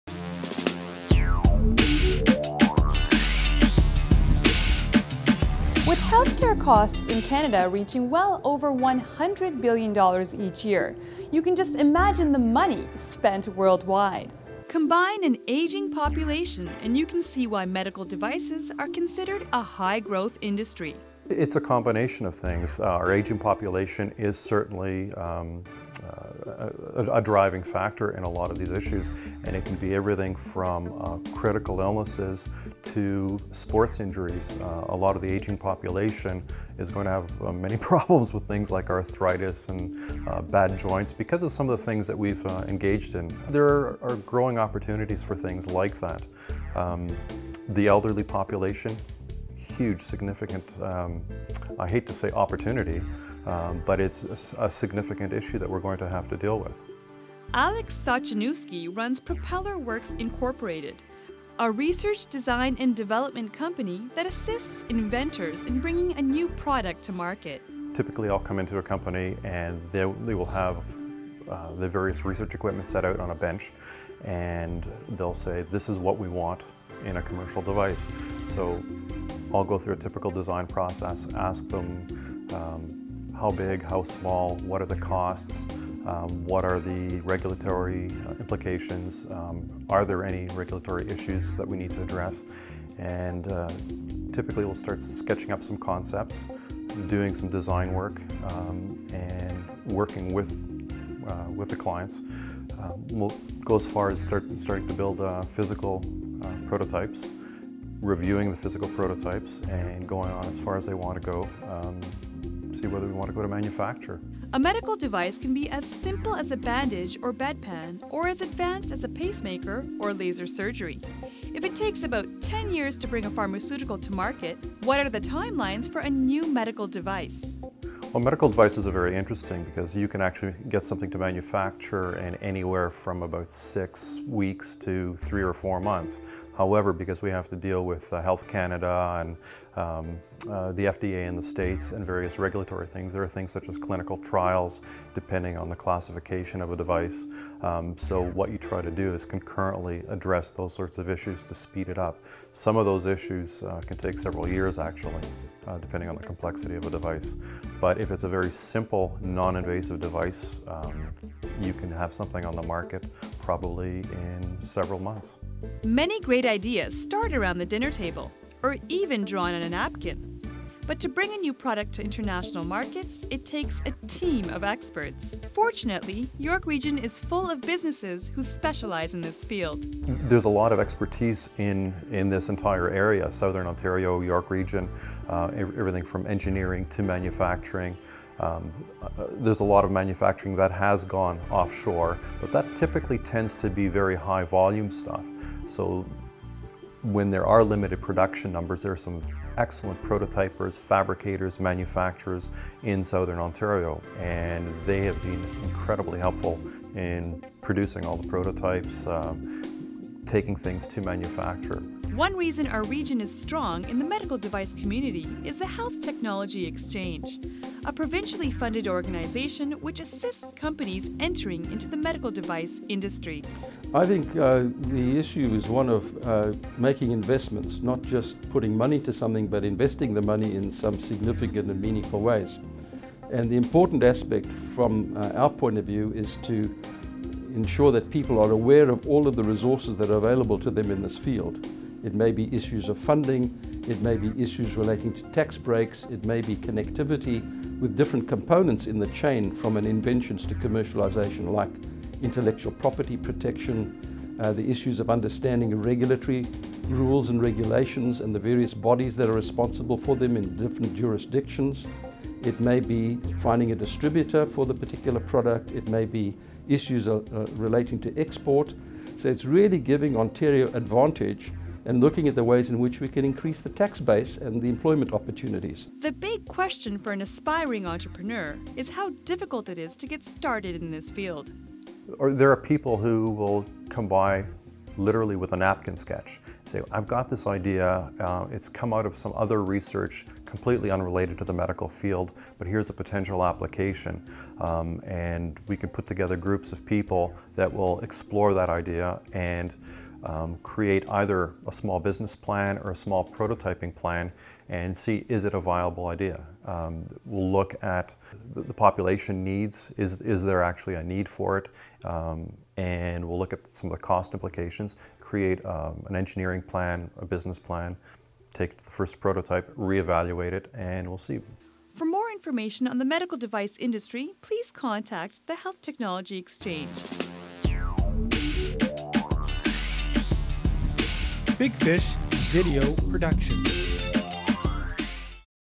Medical Devices, Alex Sochaniwskyj
With health care costs in Canada reaching well over $100 billion dollars annually, you can just imagine the money spent world wide. Combine an aging population, and you can see why Medical Devices are considered a high growth industry. for more great business videos and articles.